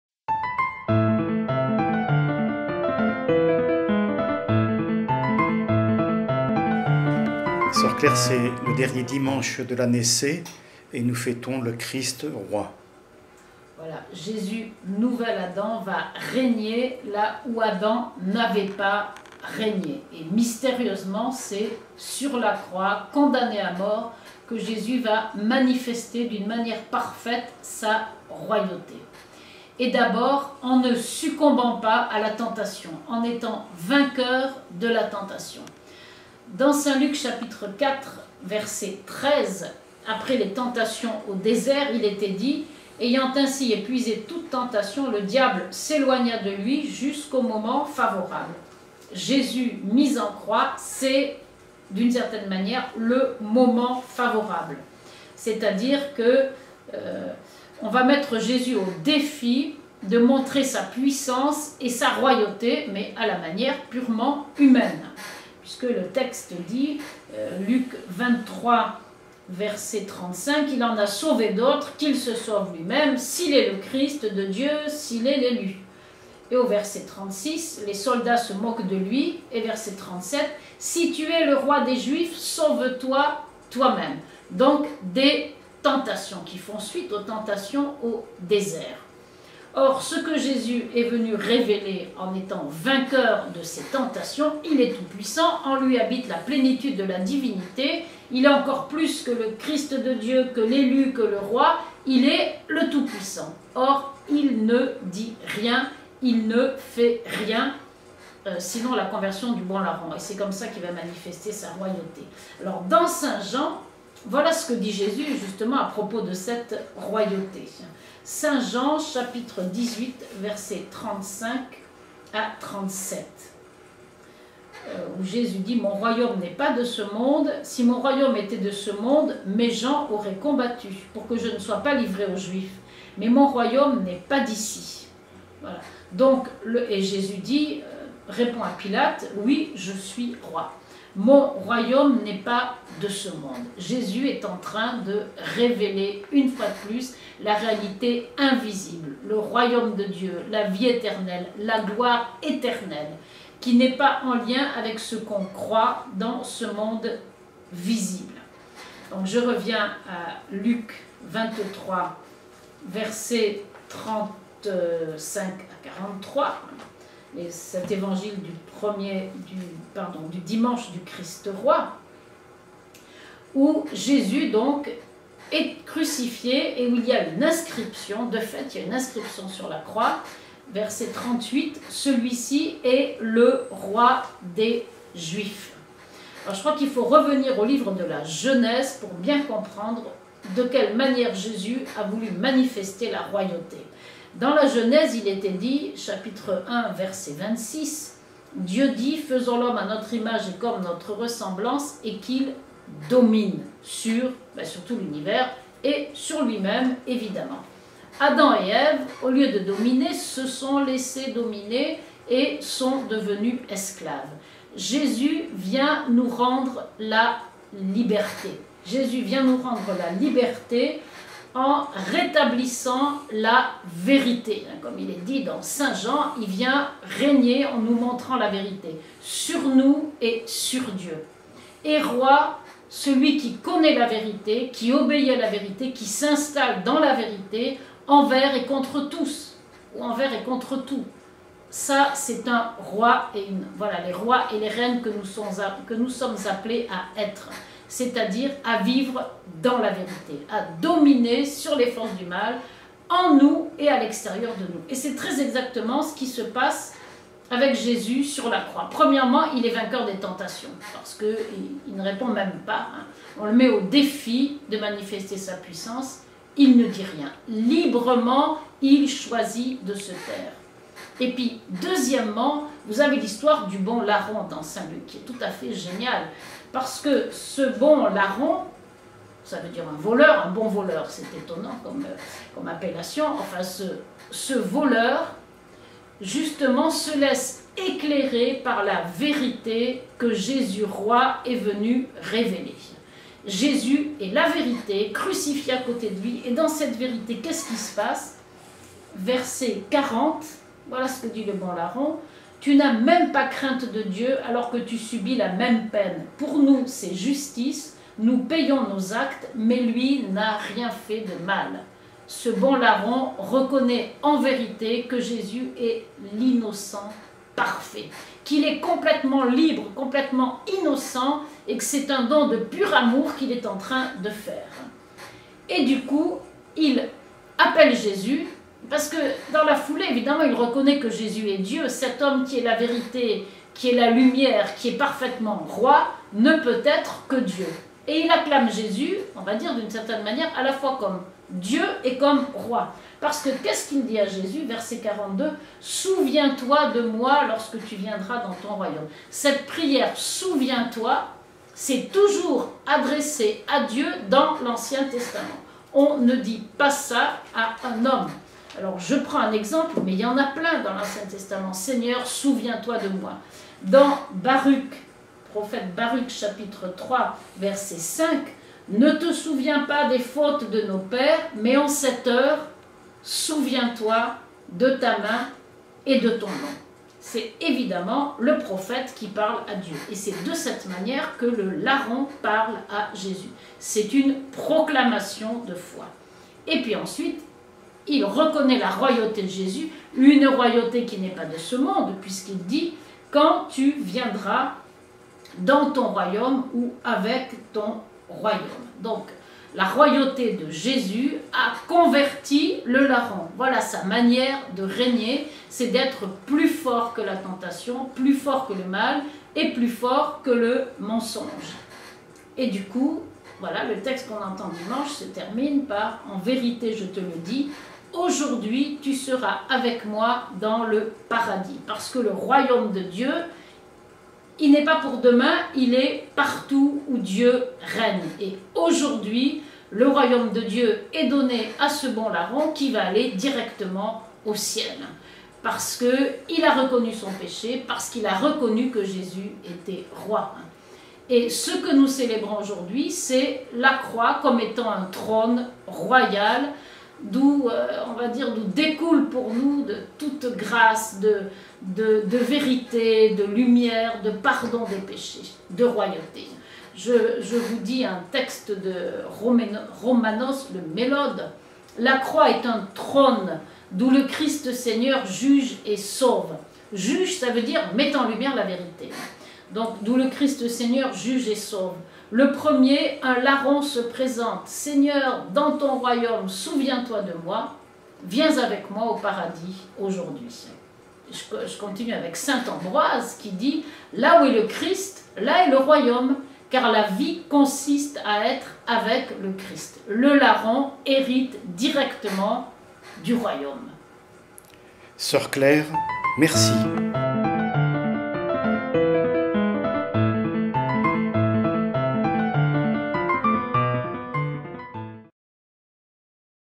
Le Christ Jésus notre Roi
Commentaire de l'évangile selon saint Luc 23,35-43